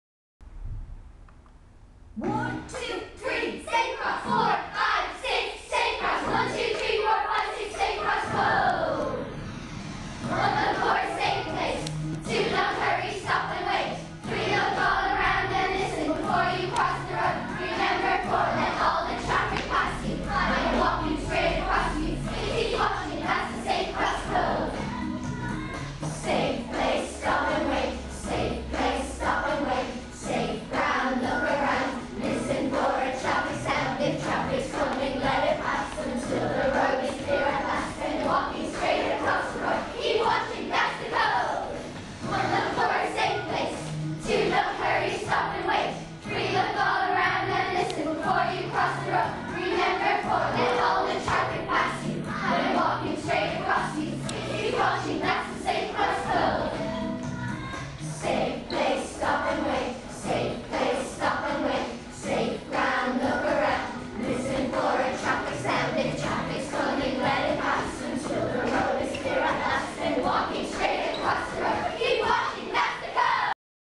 Scoil Mhuire GNS, Lucan
Ms. Foy's fifth class from Scoil Mhuire GNS, Lucan, Co. Dublin performing their Safe Cross Code routine.